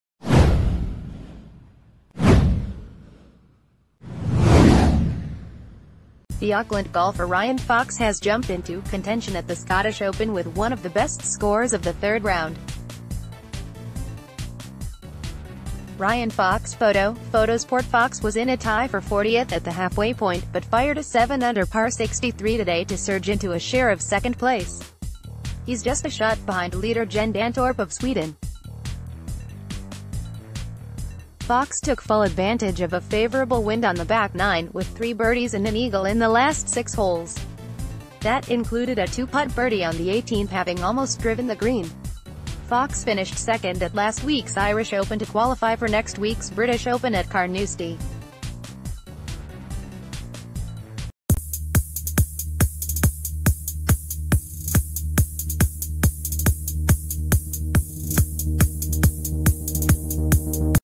Fox jumps into contention at Scottish Open
The Auckland golfer Ryan Fox has jumped into contention at the Scottish Open with one of the best scores of the third round. Ryan Fox Photo: Photosport Fox was in a tie for 40th at the halfway point, but fired a seven under par 63 today to surge into a share of second place. He's just a shot behind leader Jen Dantorp of Sweden. Fox took full advantage of a favourable wind on the back nine with three birdies and an eagle in the last six holes. That included a two-putt birdie on the 18th having al...